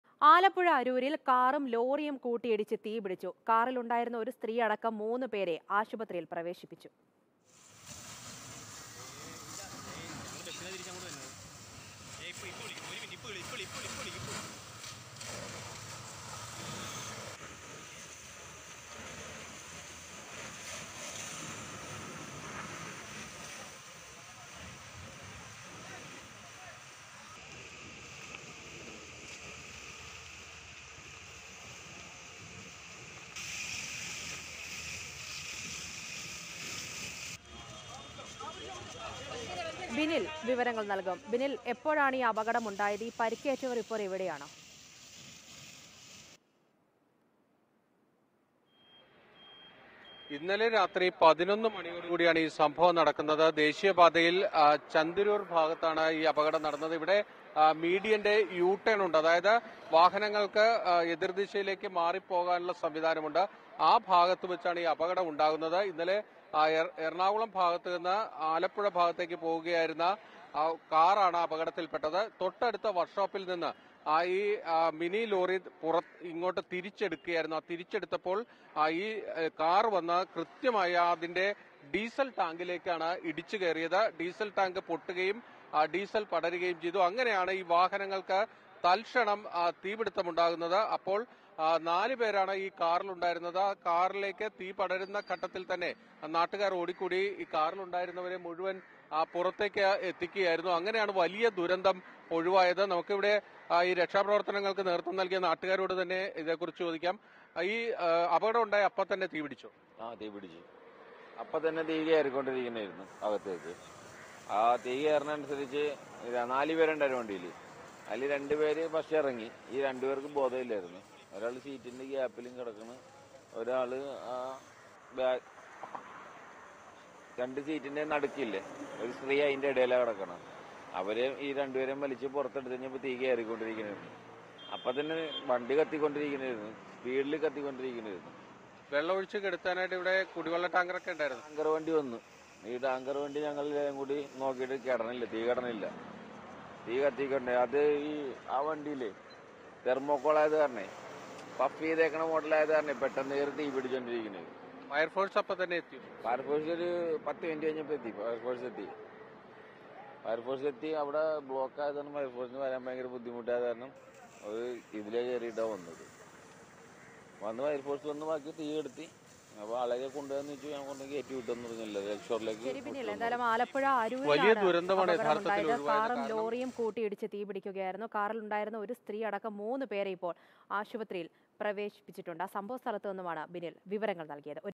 ആലപ്പുഴ അരൂരിൽ കാറും ലോറിയും കൂട്ടിയിടിച്ച് തീ പിടിച്ചു| Mathrubhumi News
ആലപ്പുഴ അരൂരിൽ കാറും ലോറിയും കൂട്ടിയിടിച്ച് തീ പിടിച്ചു. കാറിലുണ്ടായിരുന്ന ഒരു സ്ത്രീയടക്കം മൂന്ന് പേരെ ആശുപത്രിയിൽ പ്രവേശിപ്പിച്ചു.

Happy viewing!